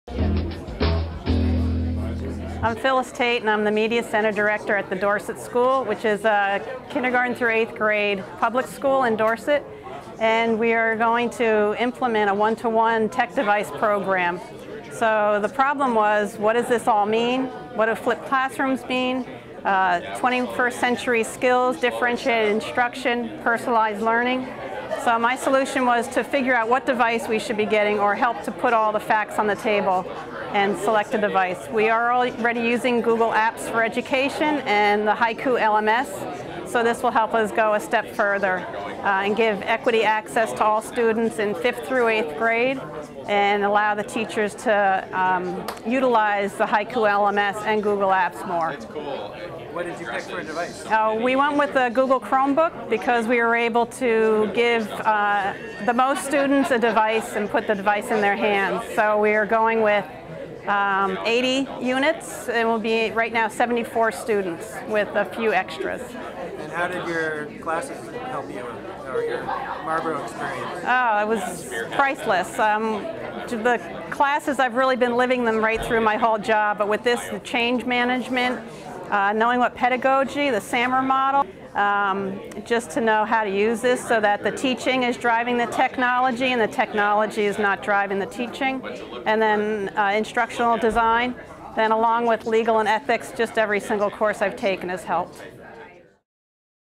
1:1 Chromebook k-12 Program - 2012 Capstone Fair. Marlboro College Graduate School, Vermont
Phyllis Tate (MAT): A 1:1 Tech Device Program.

Flipped classrooms. 21st Century Skills. Personalized Learning. Differentiated Instruction.  These are just some of the topics that are on the forefront of educational concerns today.  How does a school adapt to these changing trends?  At The Dorset School we are starting to address these issues.  We have been using Google Apps for Education with the students, teachers, and administration for five years and have implemented the Haiku LMS system.  Our next step is to implement a 1:1 Tech Device Program.  What device do we use?  How can we use these devices to their fullest potential? This is an exciting time in education and how does the school move forward in a constructive way?